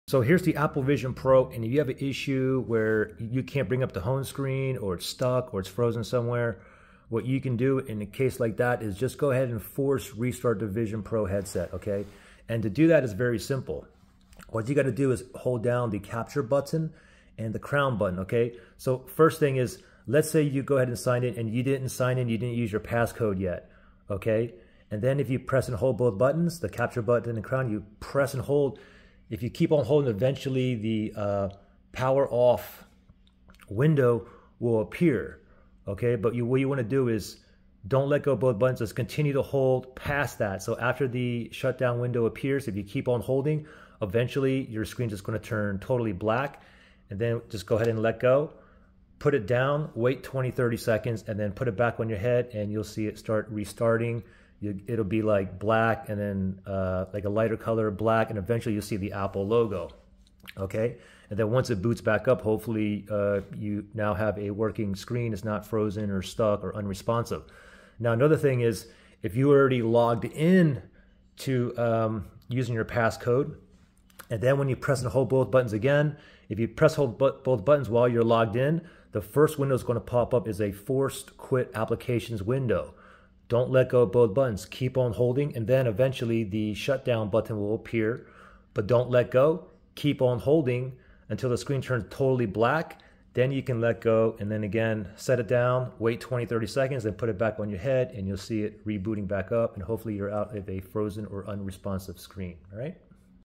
Apple Vision Pro: Frozen, Stuck or Unrespsonisve? Can't Restart? FIXED!
I show you how fix an Apple Vision Pro Headset where the screen is unresponsive, stuck somewhere or frozen or you can't do anything or proceed nor restart or shut down. You just have to force a restart. I had an issue where i saw no windows, home screen or anything and forcing a restart solved the issue. hope this helps.